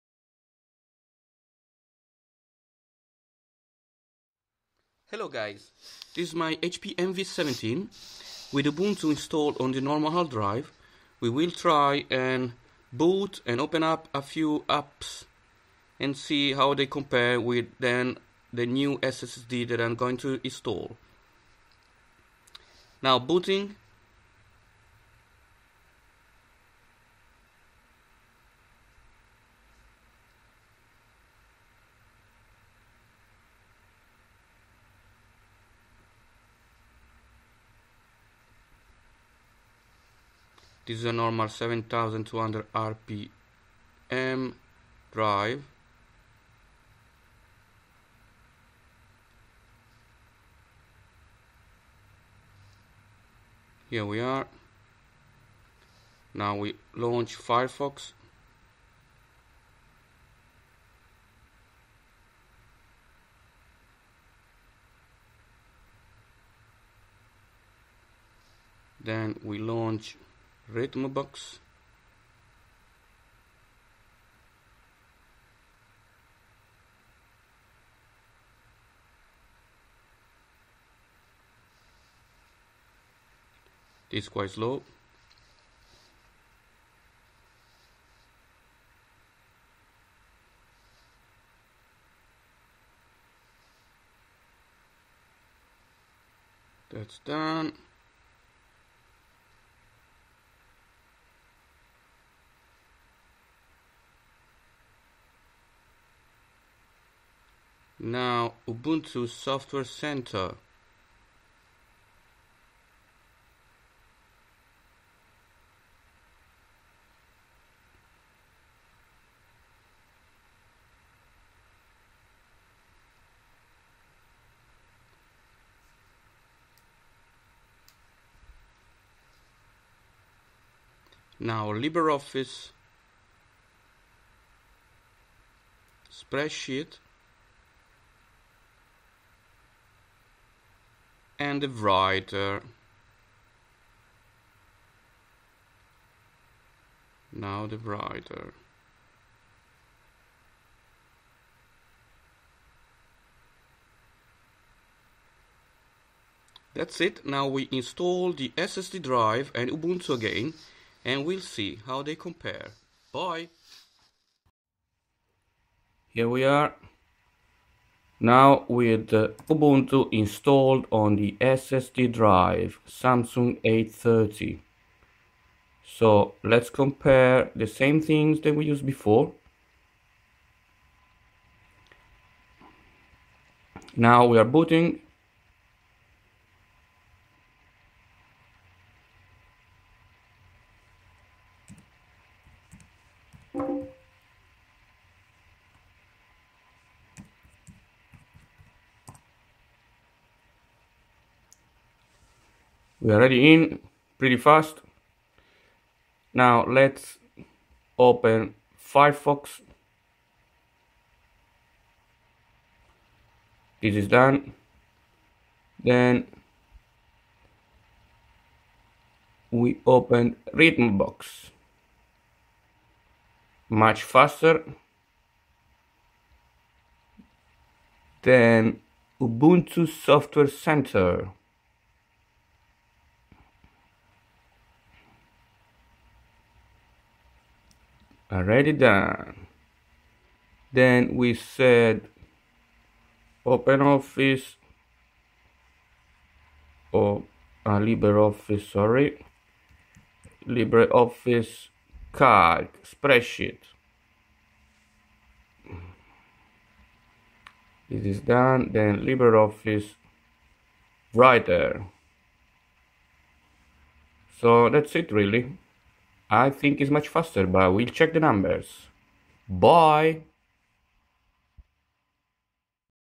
SSD VS HD ON MY HP ENVY 17 RUNNING UBUNTU
I installed a Samsung 830 SSD on my HP Envy 17 and I show the difference while booting and opening a few programs.
Recorded with my amazing Nokia 808 Pureview.